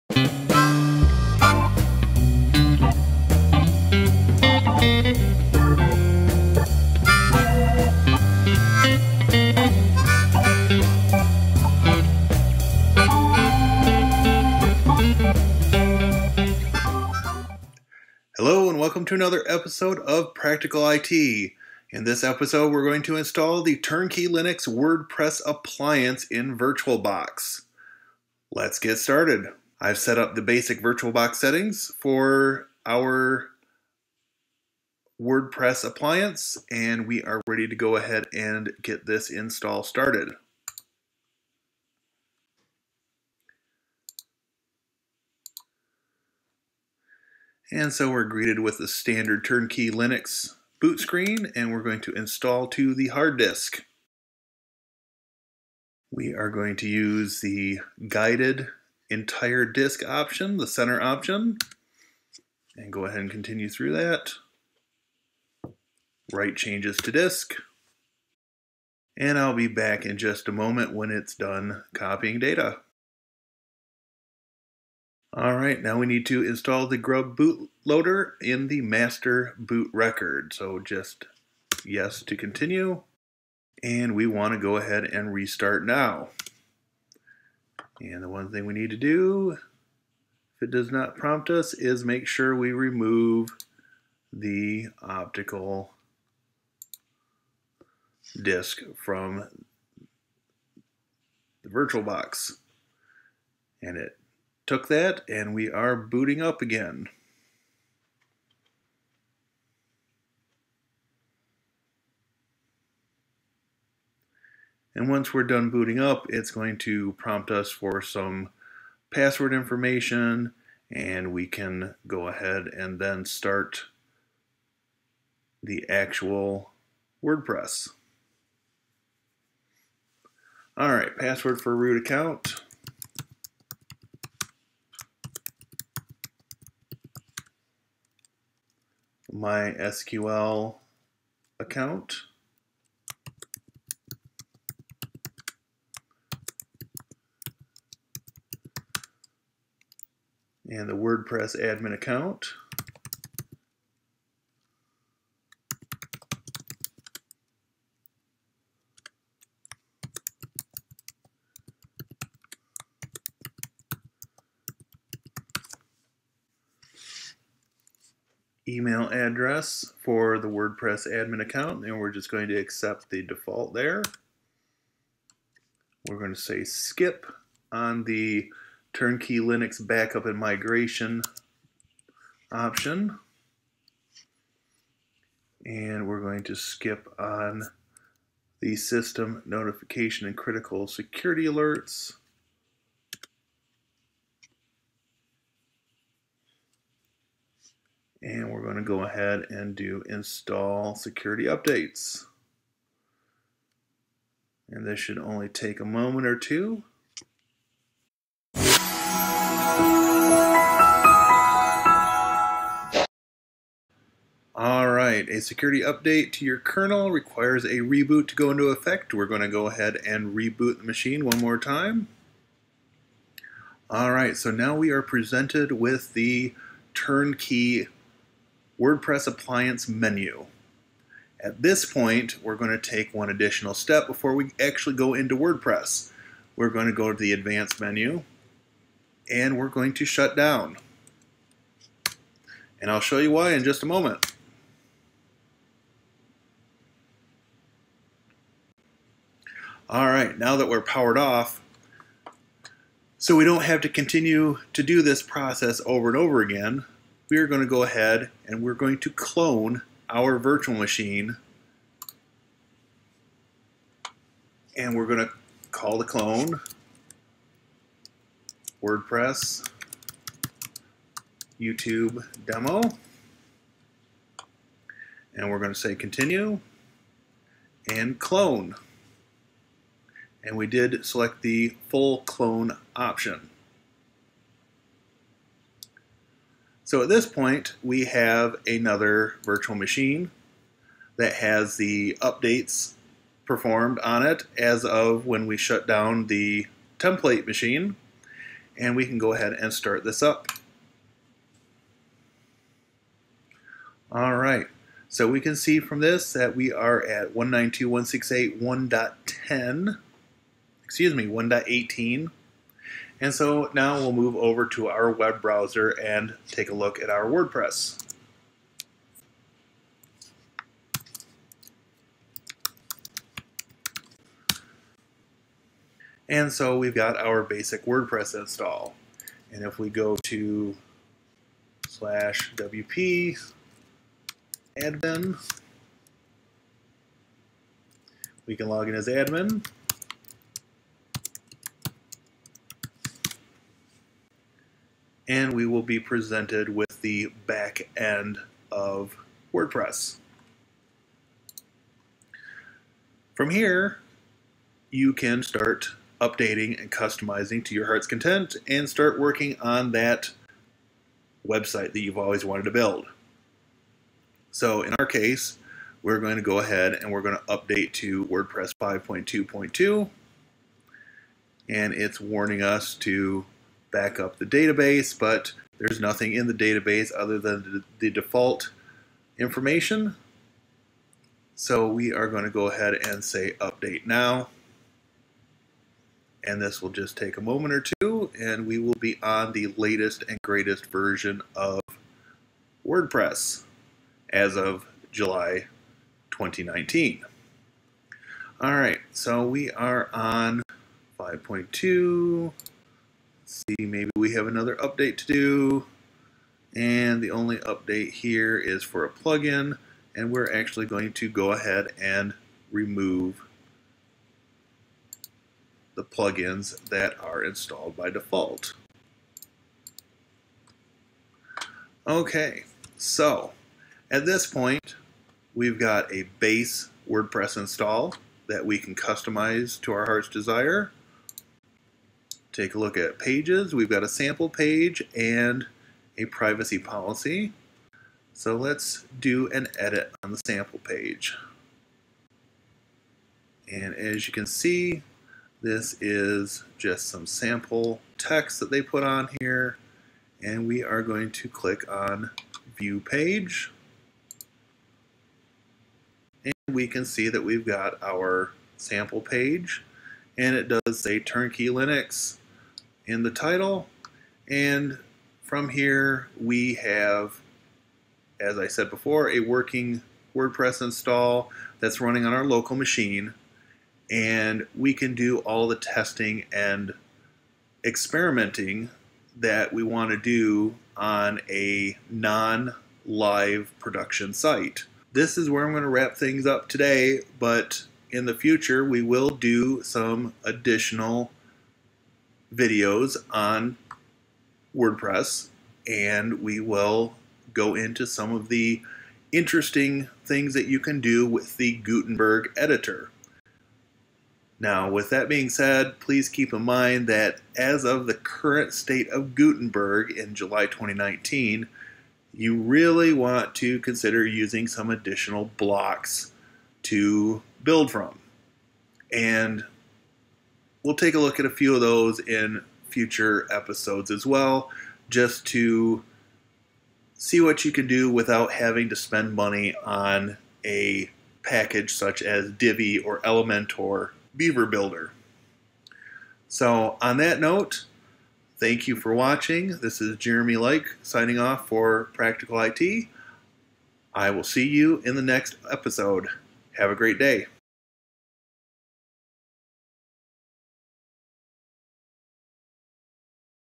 TurnKey Linux WordPress Appliance in VirtualBox
Episode 154. Install the TurnKey Linux WordPress Appliance in VirtualBox and Clone the base install so that we can create additional copies in the future.